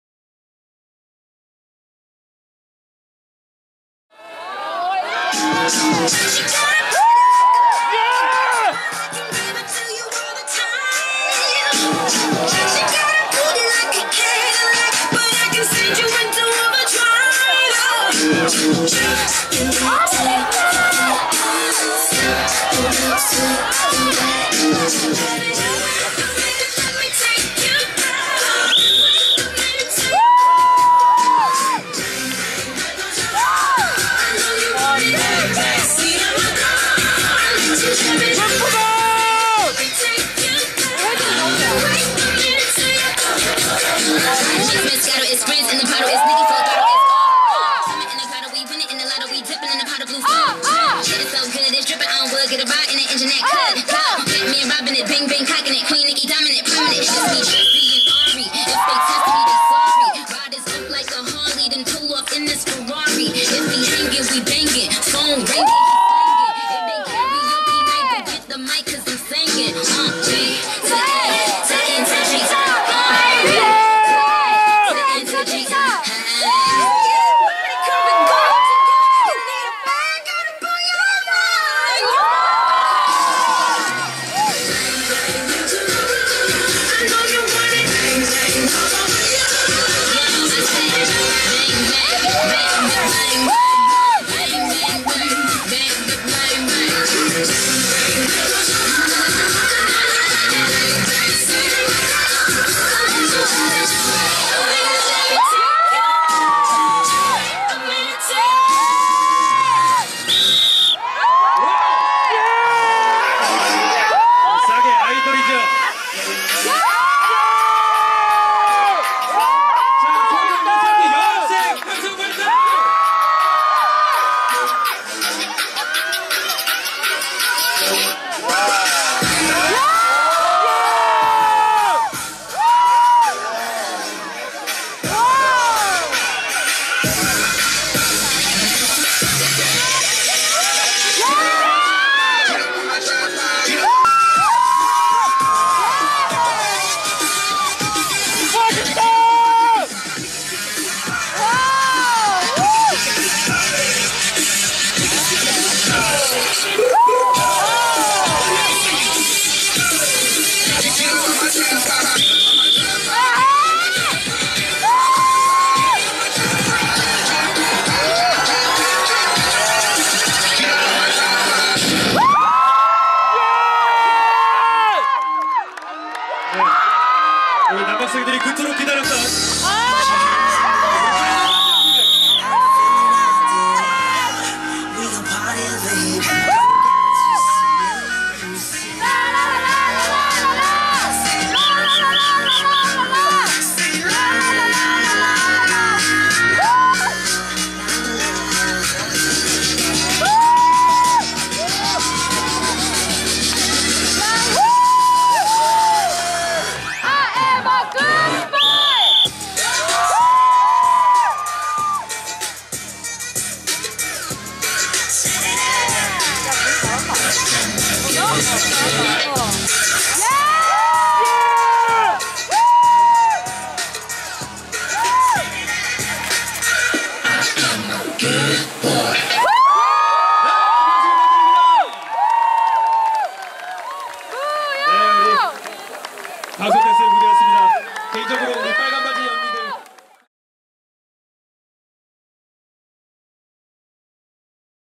[싹튜브] 서울종합예술학교 2015 체육대회 - 무용예술학부 방송댄스학과 칼군무 퍼포먼스
서울종합예술학교 2015 체육대회
무용예술학부 방송댄스학과 칼군무 퍼포먼스